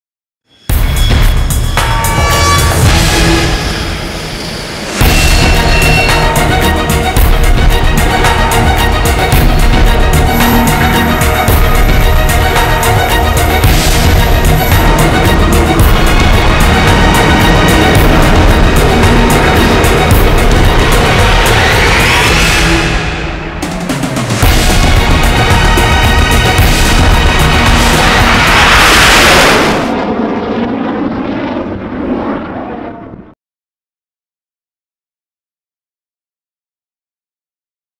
X-47B Completes First Carrier Launch
The X-47B Unmanned Combat Air System demonstrator (UCAS-D) completed its first ever carrier-based catapult launch from USS George H.W. Bush (CVN 77) off the coast of Virginia, May 14.

The unmanned aircraft launched from the deck of George H.W. Bush at 11:18 a.m. It executed several planned low approaches to the carrier and safely transited across the Chesapeake Bay to land at Naval Air Station Patuxent River, Md., after a 65-minute flight.